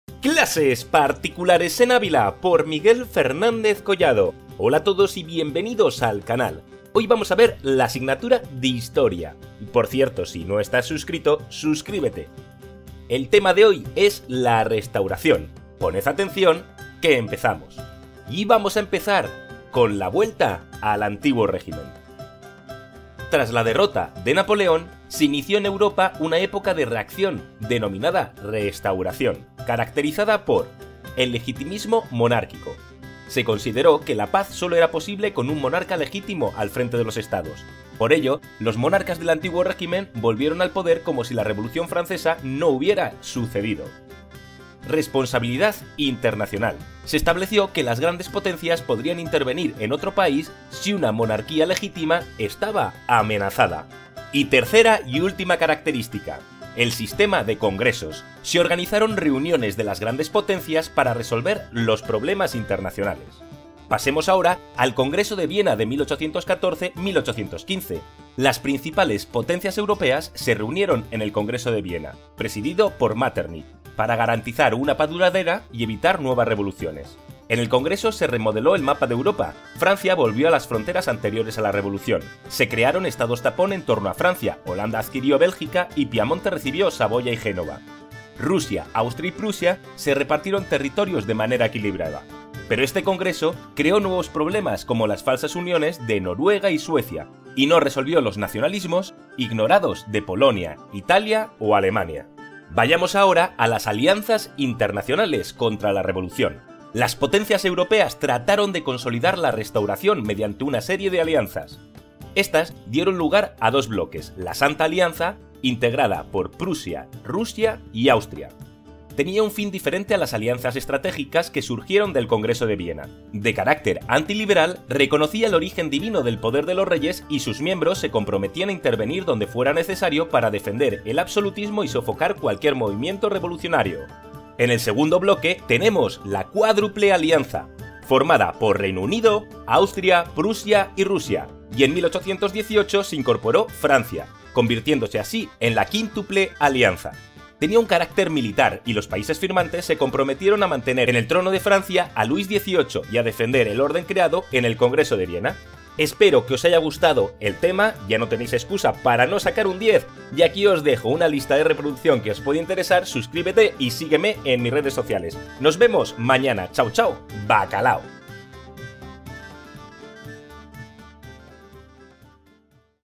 👑 La RESTAURACIÓN y el CONGRESO de VIENA
👑 En este vídeo vamos a explicar la Restauración viendo conceptos como: Vuelta al Antiguo Régimen, el congreso de Viena de 1815 y las alianzas internacionales, para alumnos de 4º de la ESO y 1º y 2º de BACHILLERATO de HISTORIA.

⭐ MÁS SOBRE HISTORIA 4º ESO y 1º de BACHILLERATO:⭐

COMPRA en Amazon con este enlace